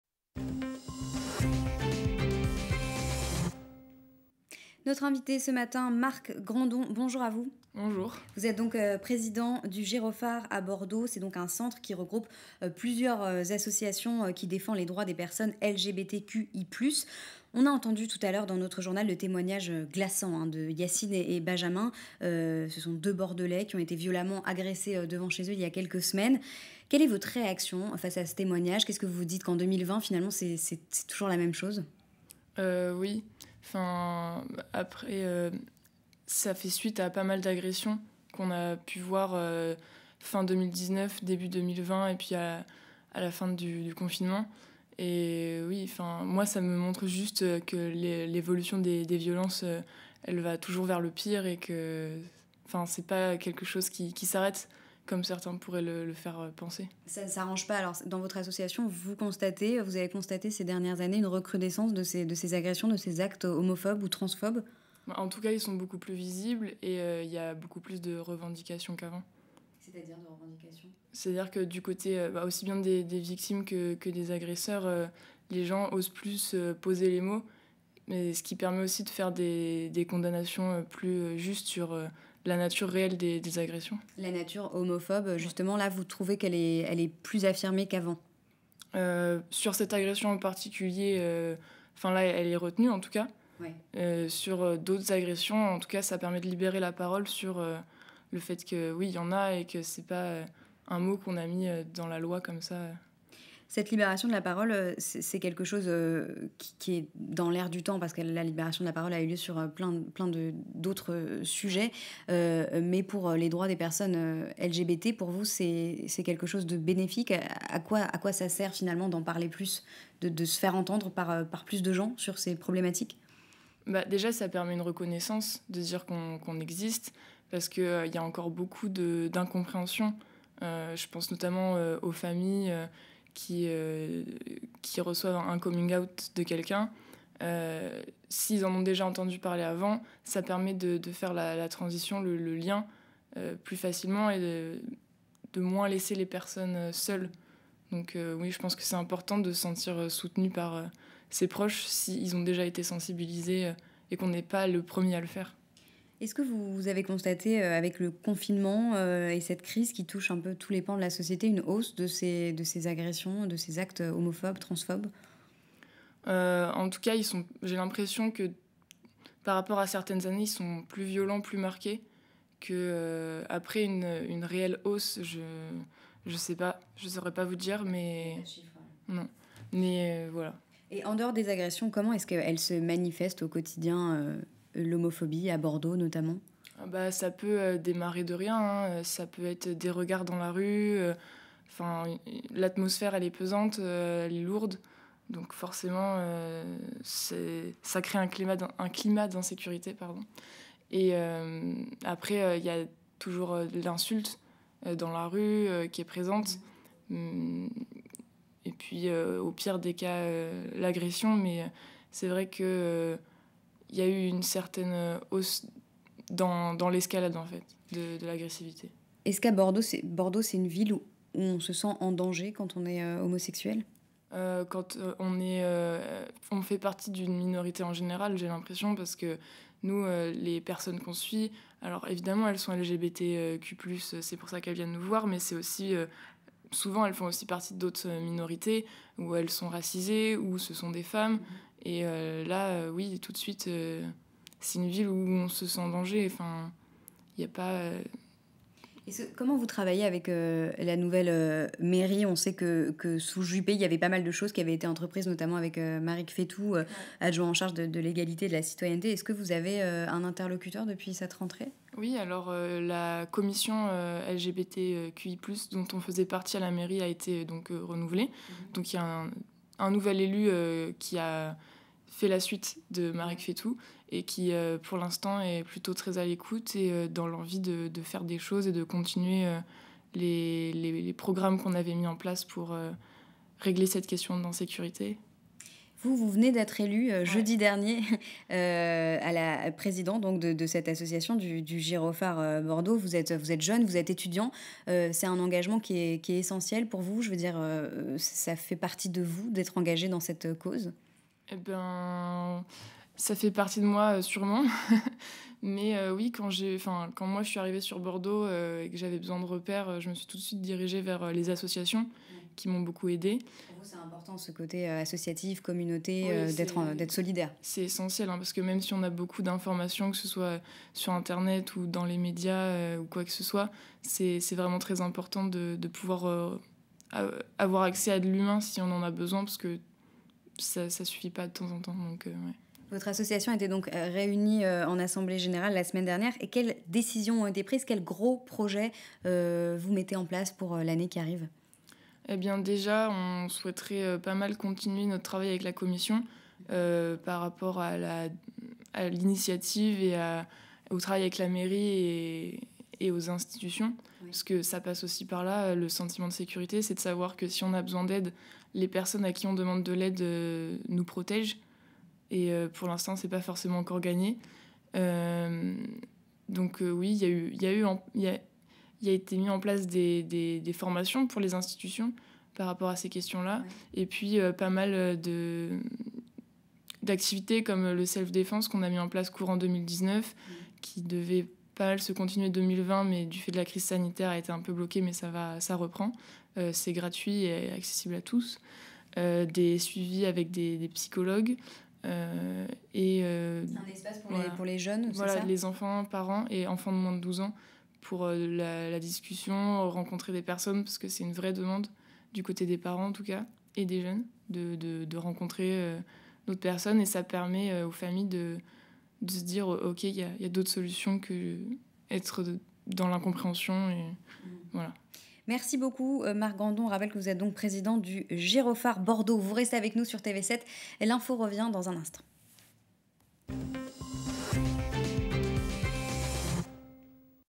La Matinale | L'invité | Marc Grandon, président du Girofard
Homophobie : quelles actions à Bordeaux ?